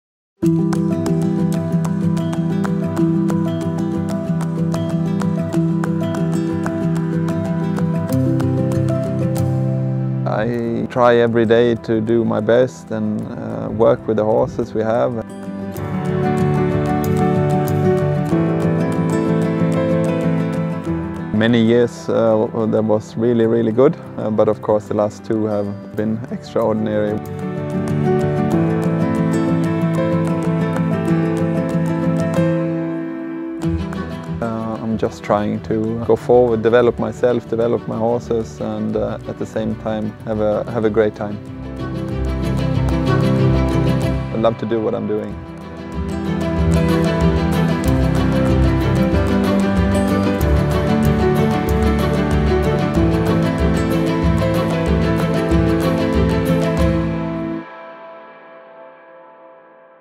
Training day with Henrik von Eckermann | 2022
“I try every day to do my best [...]
I’m just trying to go forward, develop myself, develop my horses and at the same time have a great time.”
Henrik von Eckermann.